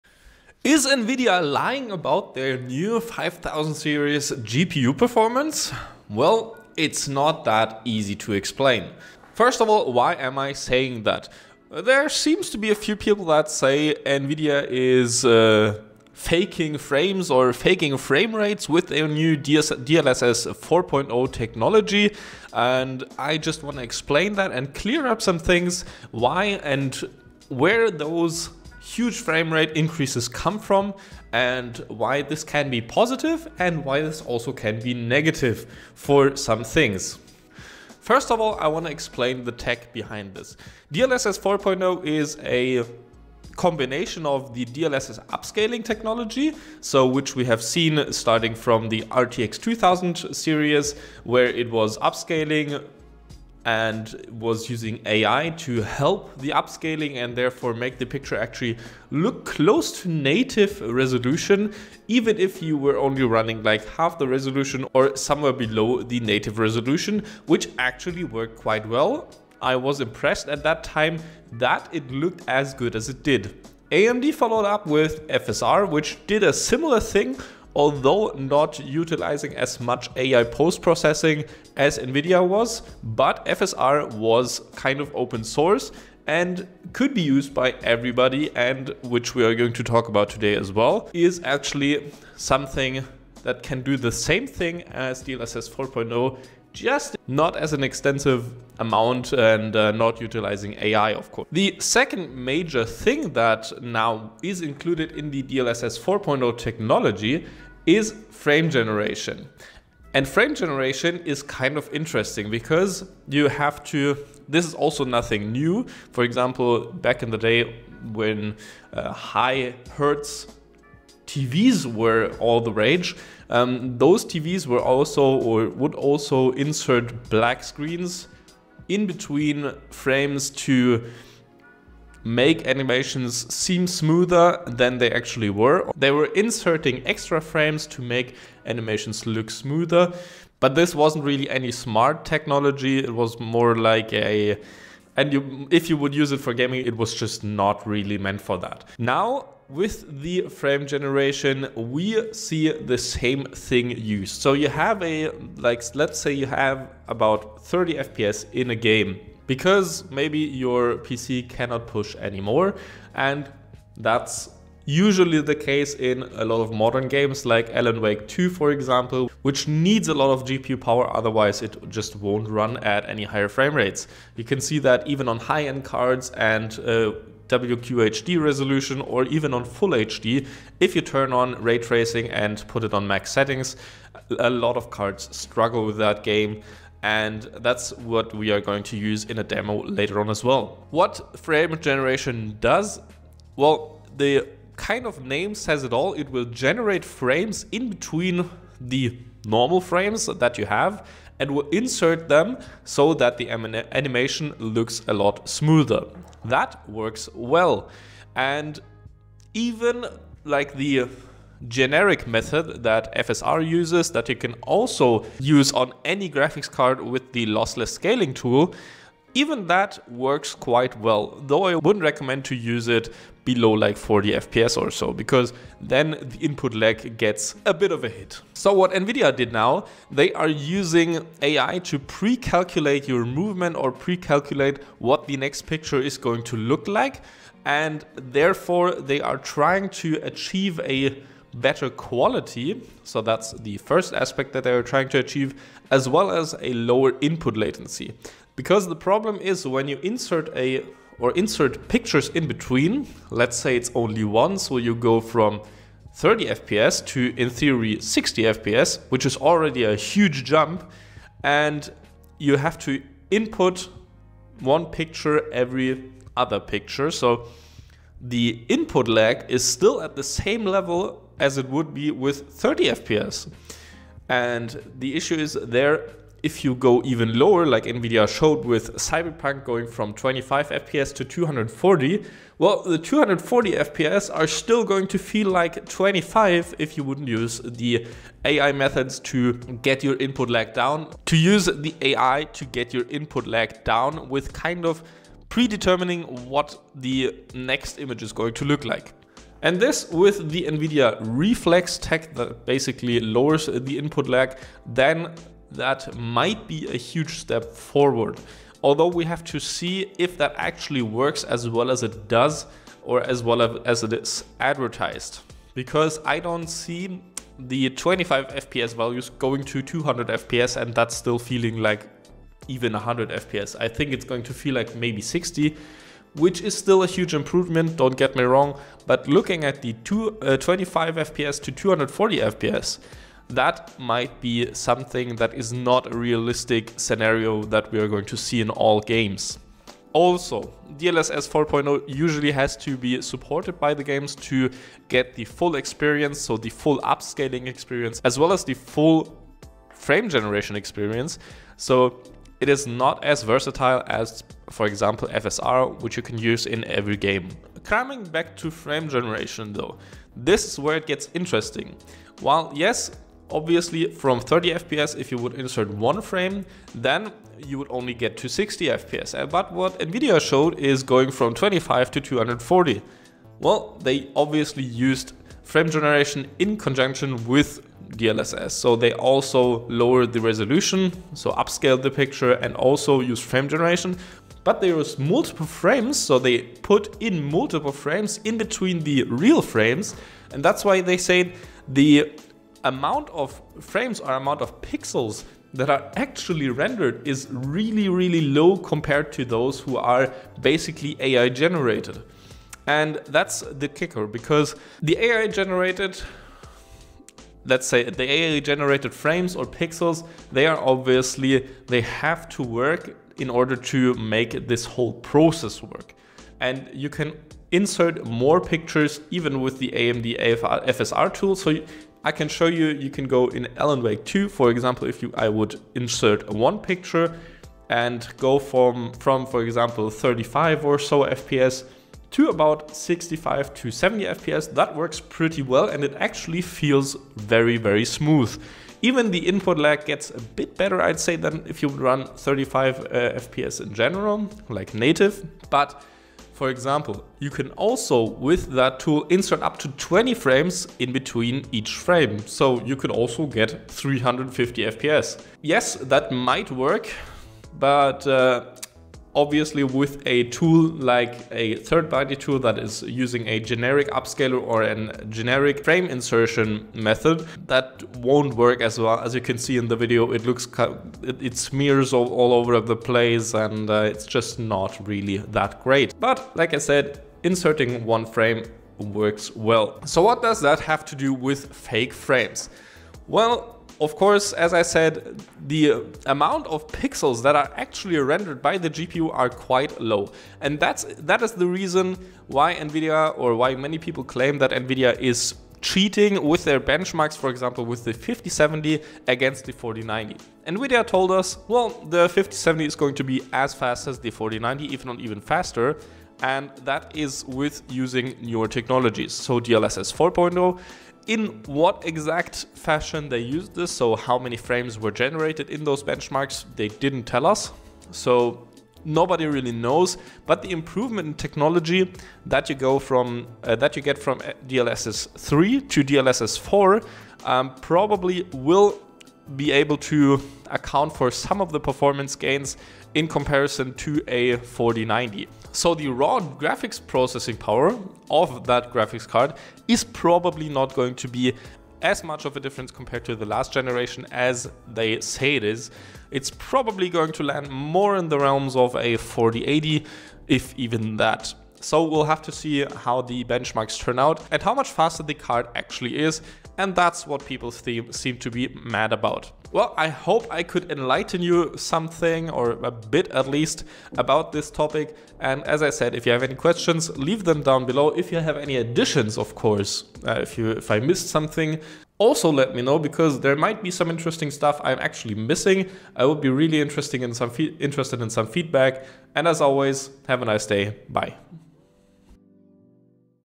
Is Nvidia Scamming Gamers? Adressing "Fake Frames" | Explaining DLSS 4.0 and Frame generation + Test
People are claiming that Nvidia is scamming gamers out of performance wirh their new RTX 5000 Series, Claiming their RTX 5070 reaches Performance levels of the previous RTX 4090 with DLSS 4.0 and Frame generation. Today i want to discuss what these technologies do and how they can impact gaming performance for anyone